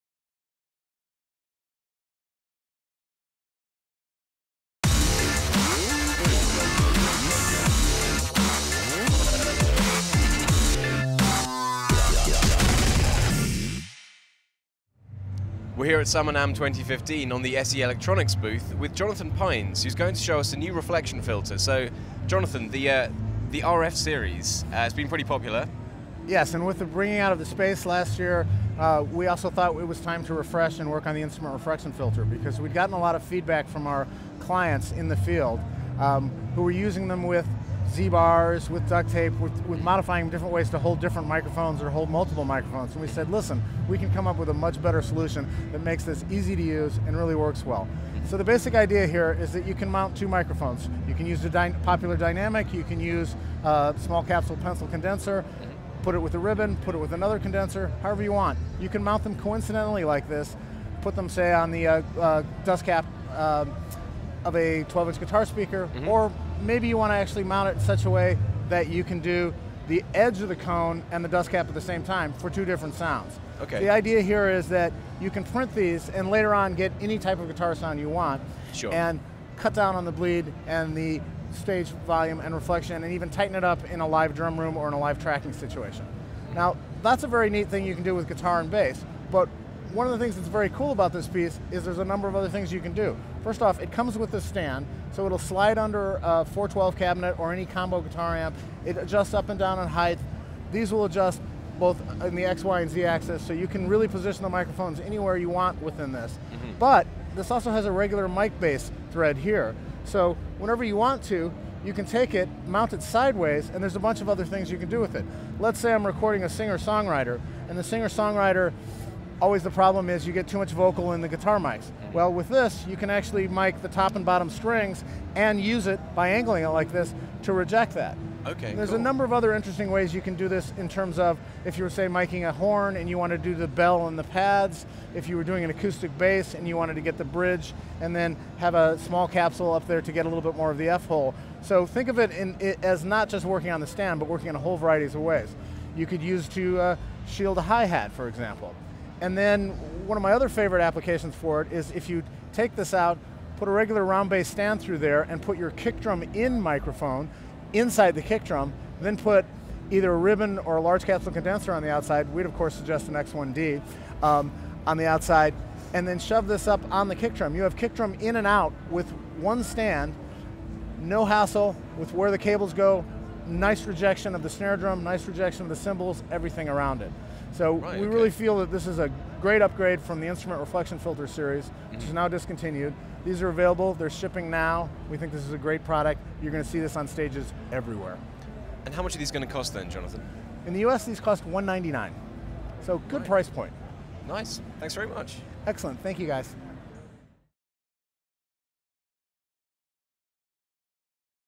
SE GuitaRF - Summer NAMM 2015
The guitaRF acts as a Reflexion Filter, an isolator and a double mic stand